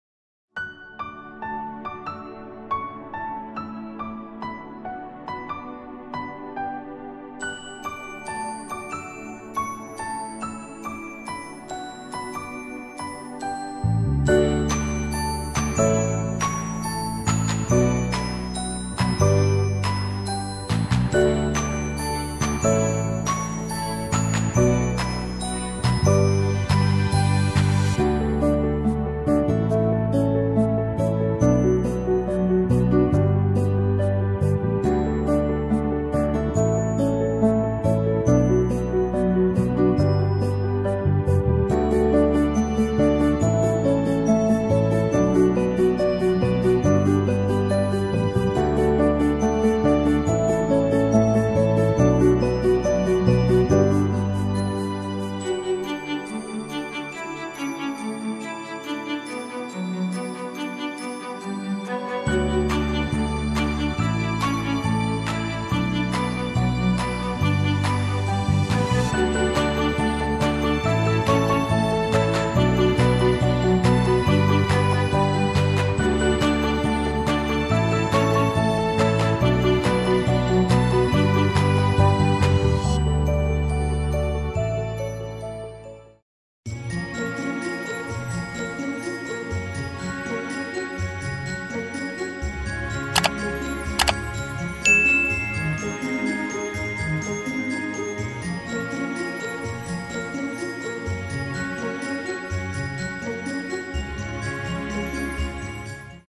ProShow Producer Merry Christmas
Проект ProShow Producer Merry Christmas
Сделан на основе проекта After Effects в программе ProShow Producer 6 .